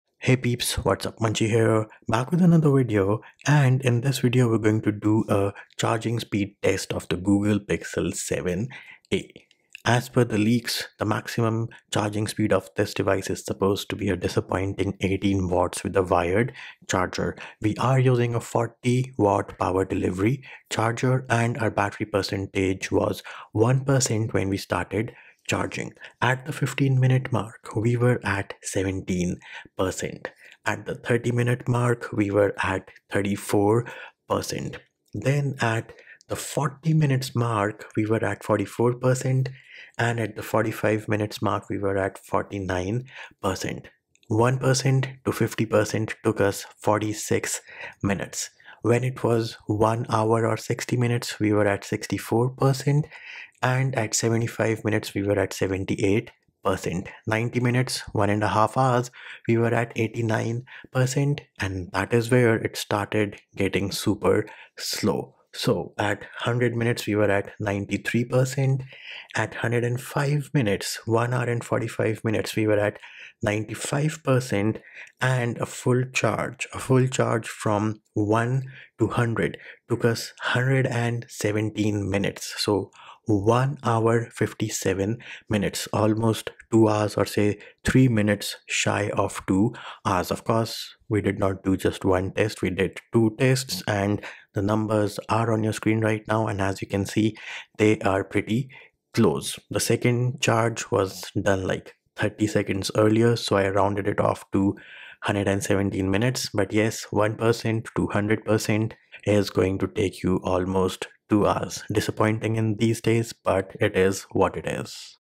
Google Pixel 7a Charging Speed Test!
Do you want to know how fast the Google Pixel 7a charges? In this detailed video we will do a complete charging test of the Google Pixel 7a from 1 to 100%.